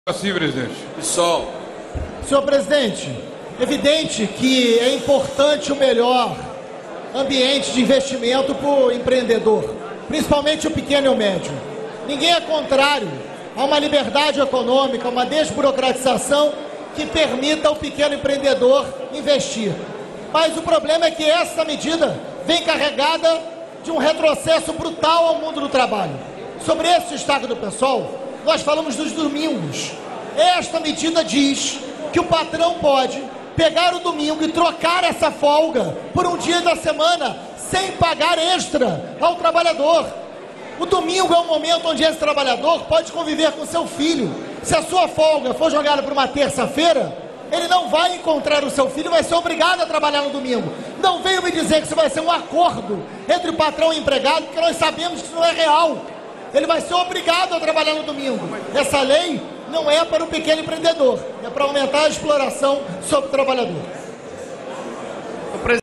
Freixo comenta MP da Liberdade Econômica
MP da Liberdade Econômica: Freixo diz que estimulo à economia não pode sacrificar direitos dos trabalhadores